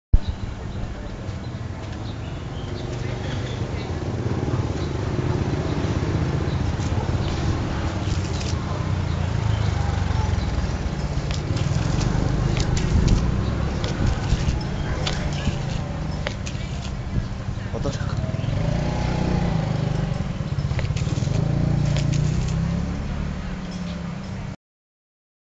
4L tank Water gun made in Thailand
Shooting about 30 feet without wind effect.
The tank volume is 4 liter.
It is not pumping action type. So you can splash quickly and many times.